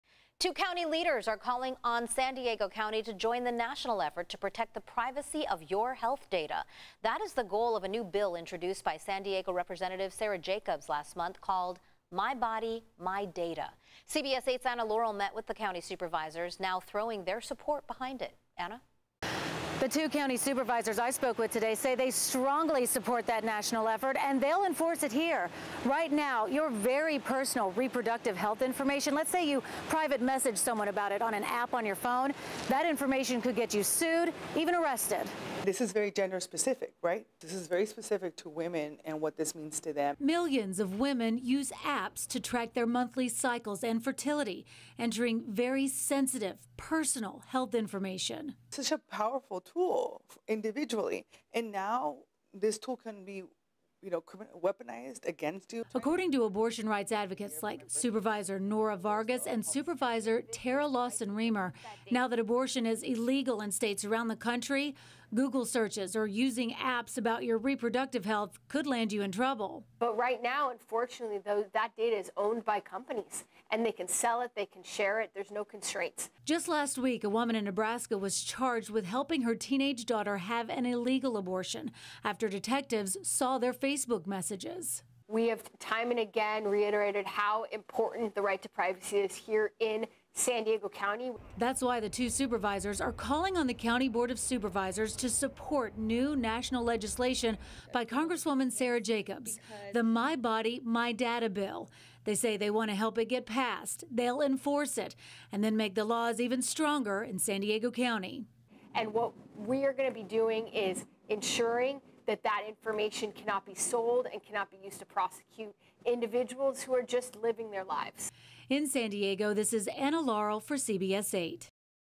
'My Body, My Data' | The fight to protect reproductive health information
Authors of the bill said your reproductive health information can be sold to the highest bidder, turned over to the government and weaponized against you.